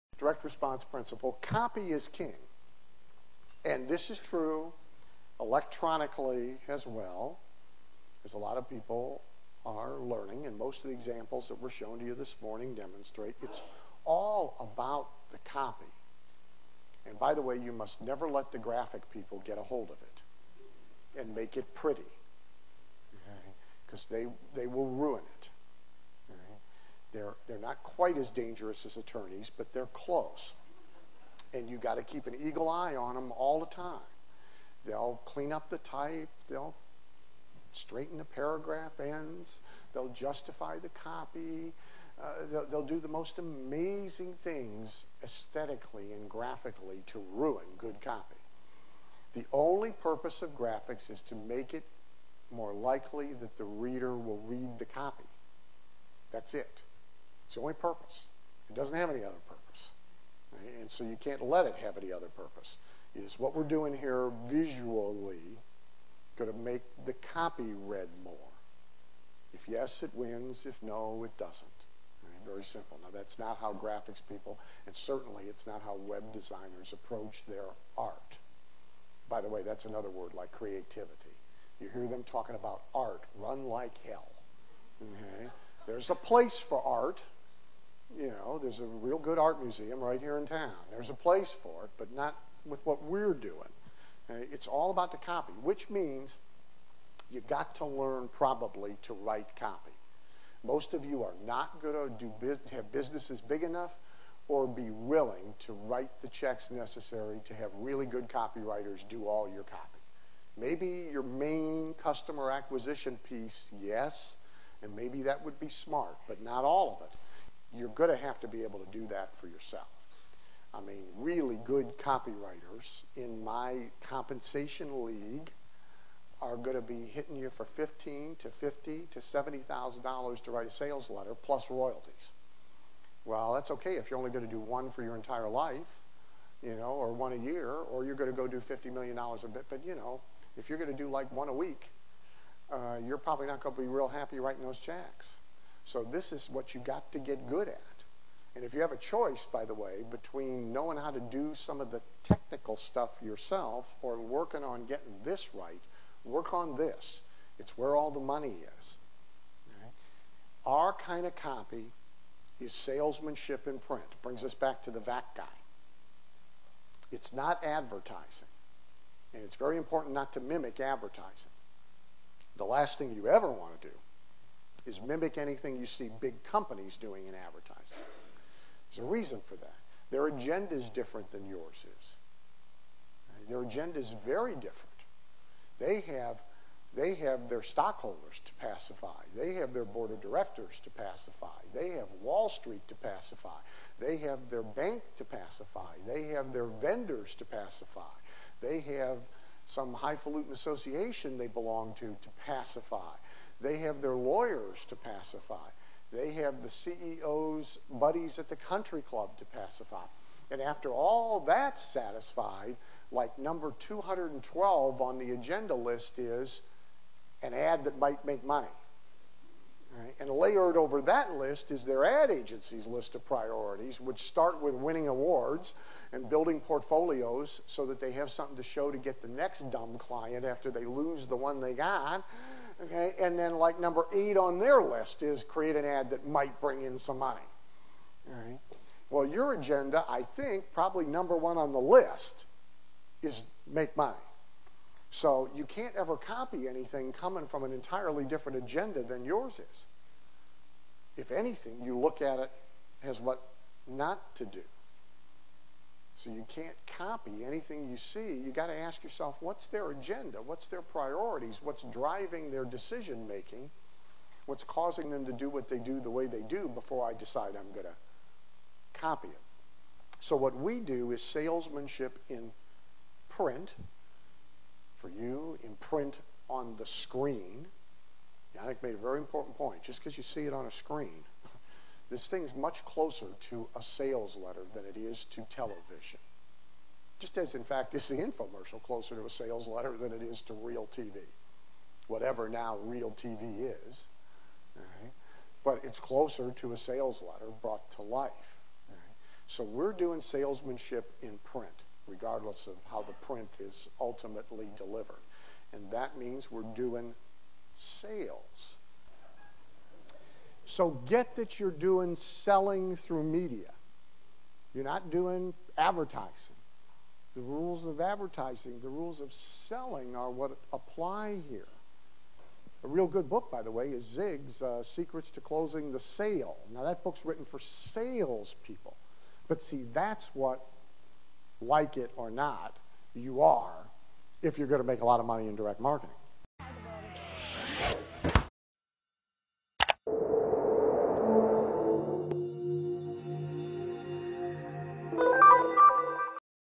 Dan Kennedy Live: Copy is King
Dan Kennedy, the grandmaster of information marketing, shares his insights into the challenges and rewards of the business of selling information with attendees at the System Seminar.